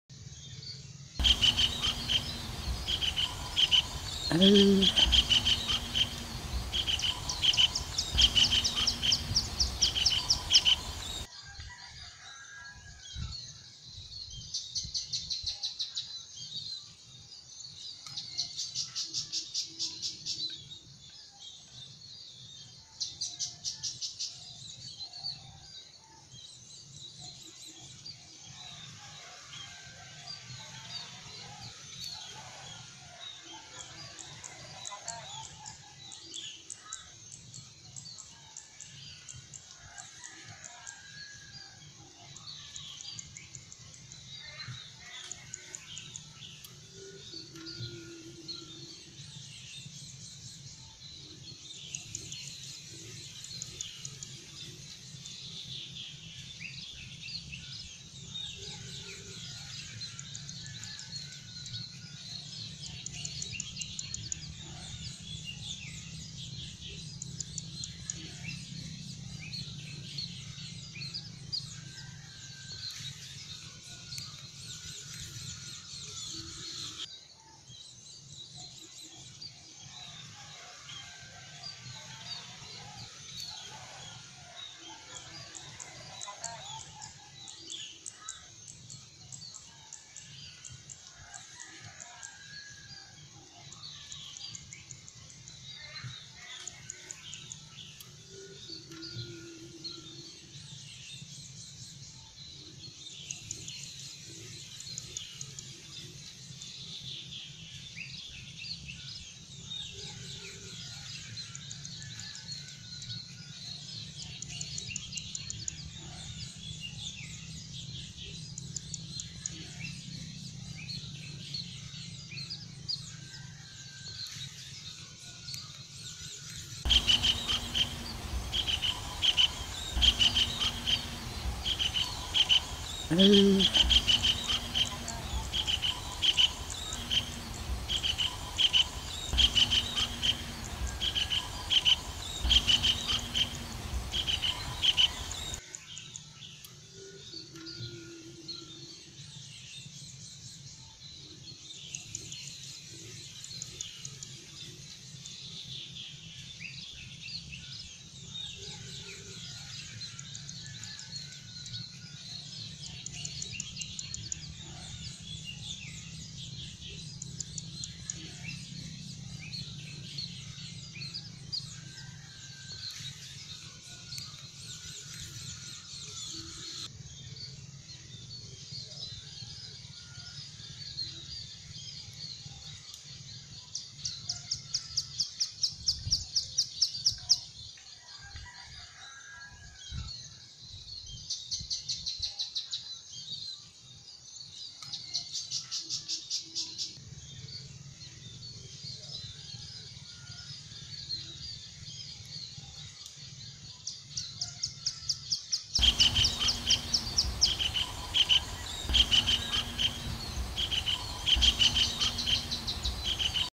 Suara burung tengkek cekakak jawa ditengah sawah sedang mengintai mangsa
Burung cekakak jawa warna biru biasa hidup di pinggir sungai sehingga sering juga disebut burung cekakak sungai. Makanan utama burung ini adalah udang tapi binatang lain seerti katak siput dan kecoa bisa jadi mangsanya. Sehingga banyak yang menyebutnya dengan nama raja udang . burung ini punya suara yang keras yang identik dengan suara cekakak maka banyak orang yang menamakan  burung cekakak